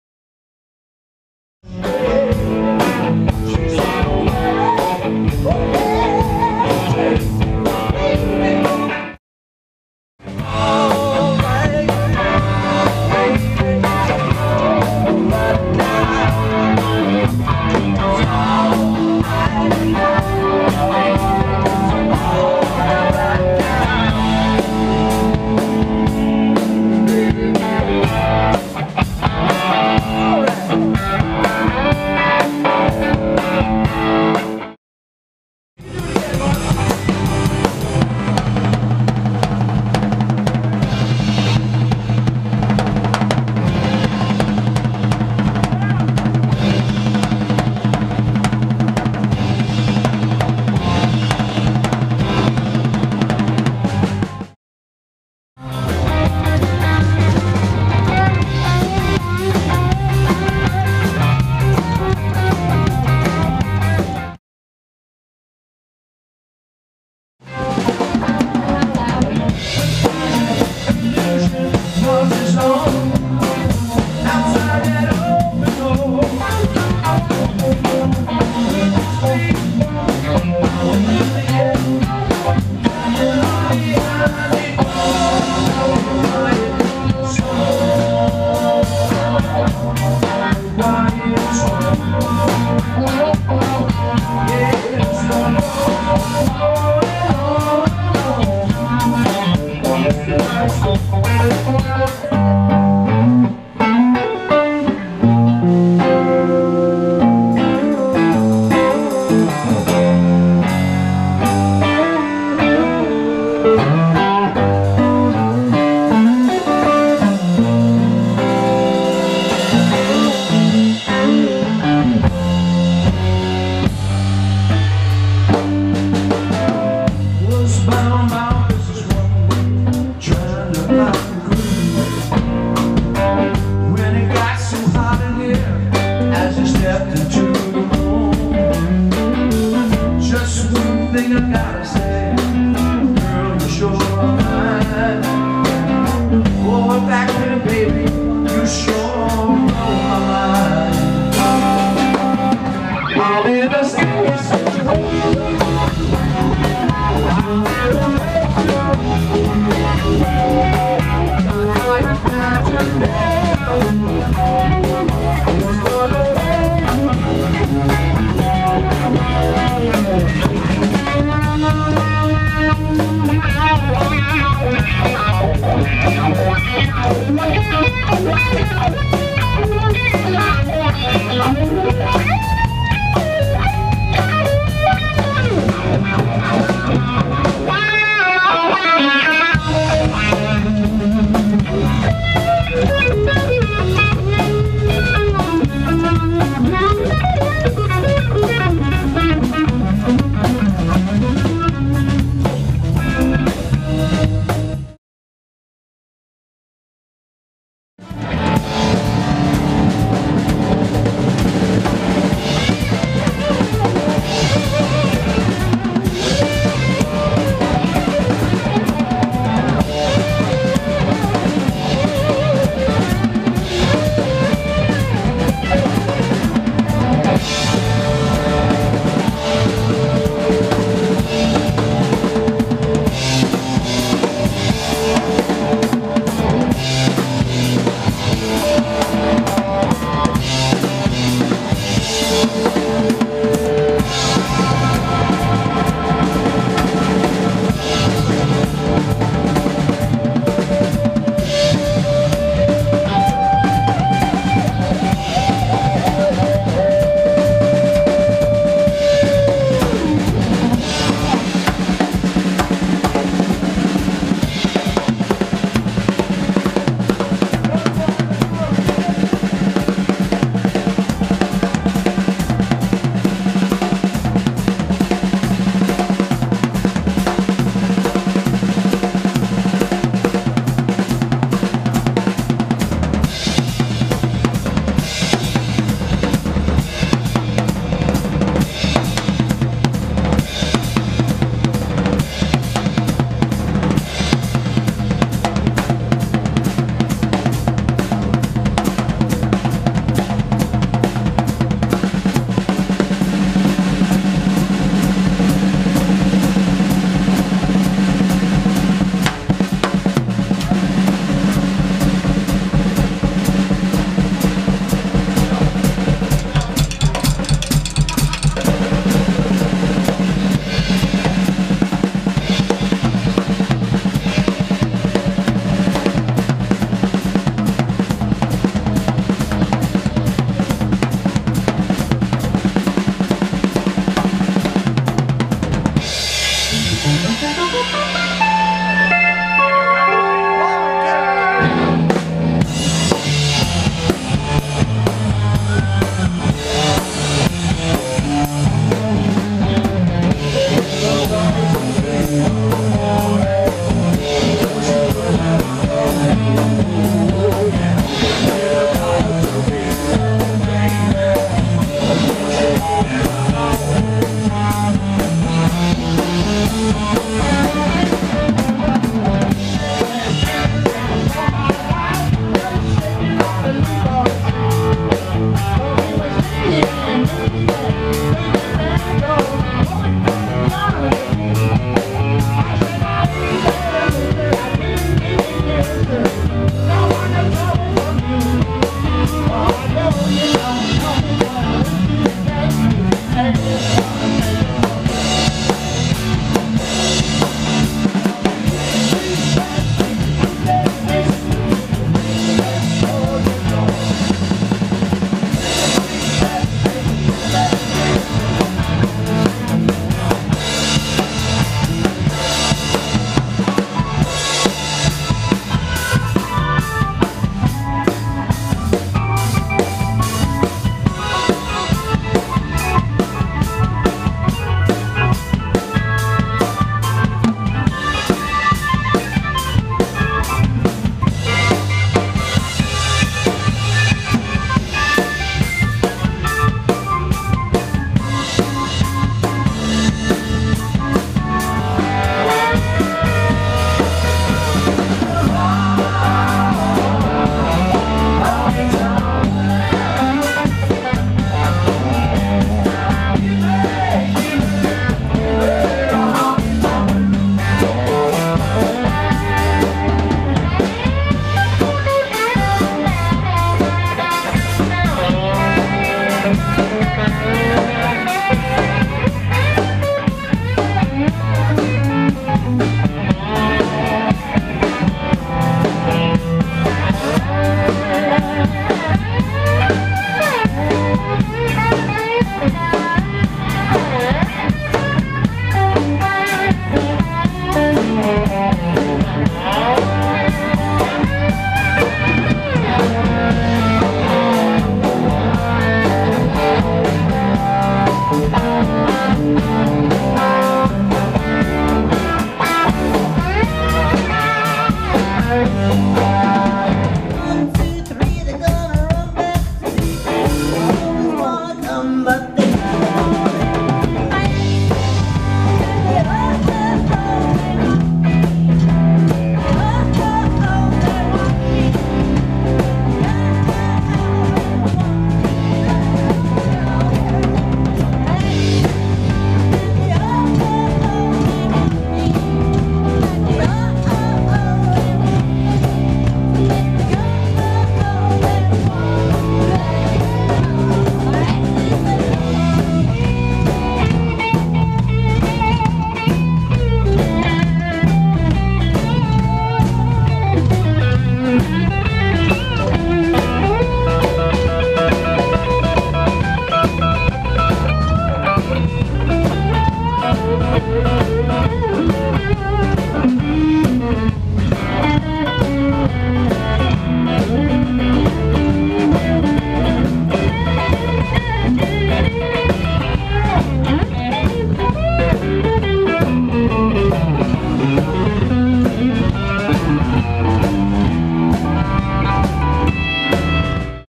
Bob Noble and Blue Voodoo Medley - March 2017
Bob Noble and Blue Voodoo - a hard working 3 piece  band from Central PA, playing both originals and covers.  Check them out on the web at bobnobleandbluevoodoo.com